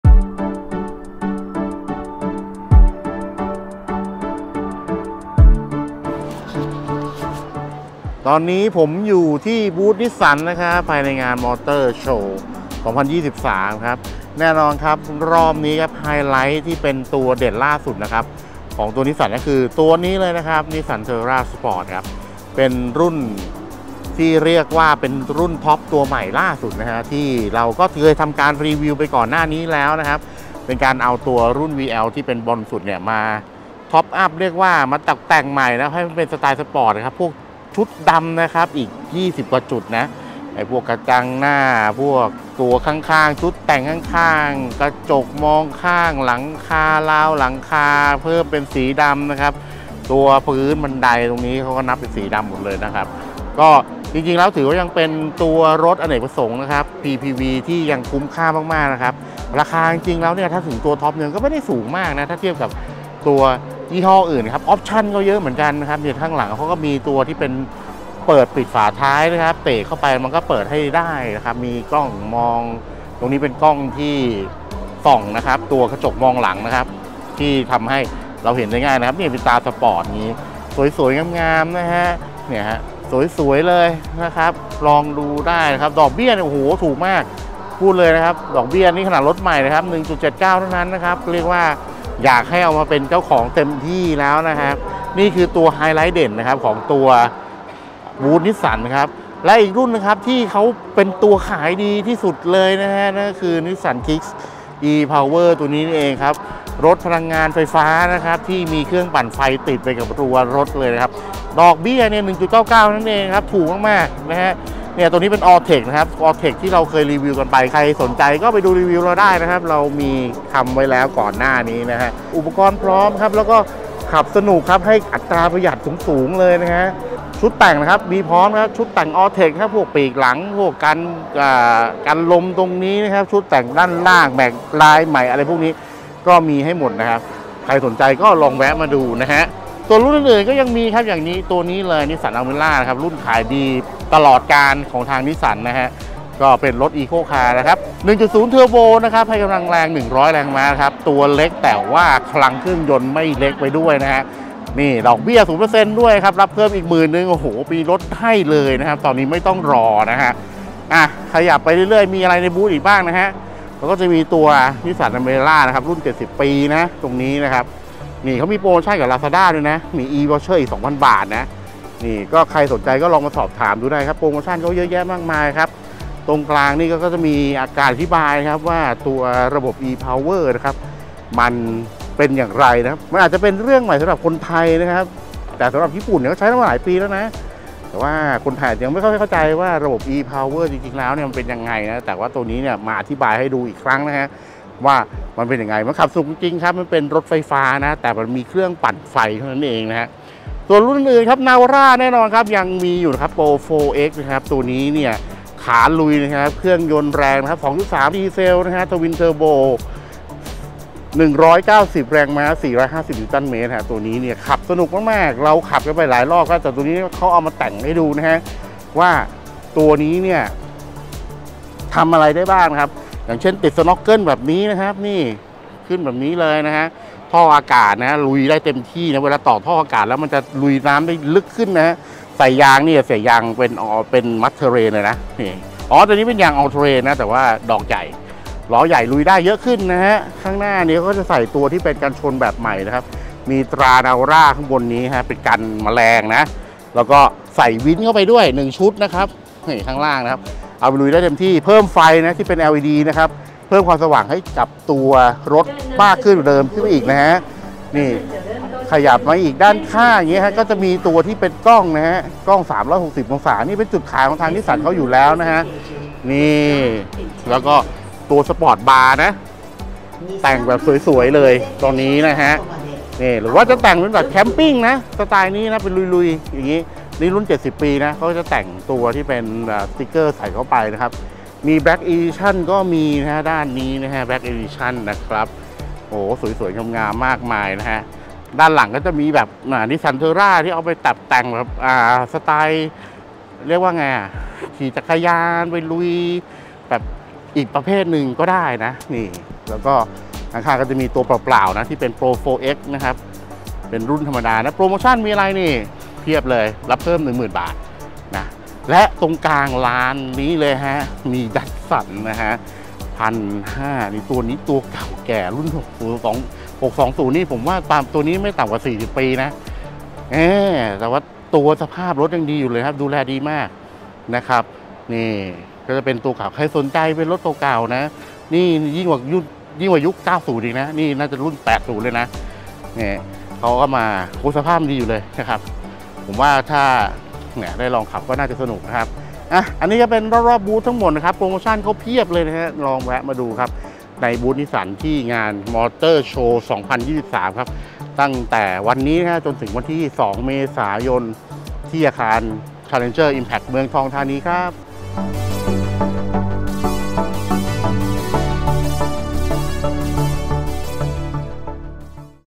เดินชมรอบบูธ Nissan ที่งาน Motor Show 2023
คลิปนี้ EARTH AUTODEFT จะพาทุกคนไปเดินรอบบูธของ Nissan โดยตัวเด่นสุดในบูธคงหนีไม่พ้น Nissan Terra Sport รุ่นย่อยตัวท็อปใหม่ที่เพิ่งเปิดตัวไปได้ไม่นานนี้ รวมทั้ง Nissan Kicks e-POWER รถไฟฟ้าพร้อมเครื่องยนต์ปั่นไฟ ที่ทำยอดขายได้ถล่มทลาย ที่มาพร้อมโปรโมชั่นสุดพิเศษ ภายในบูธครั้งนี้ที่งาน Motor Show 2023 จะมีอะไรบ้าง ติดตามชมกันได้เลย

อ่าน นิสสันยกทัพใหญ่ลุยงาน Motor Show 2023 นำโดยทัพหน้า Nissan Navara Sport และ Kicks e-POWER ได้ที่นี่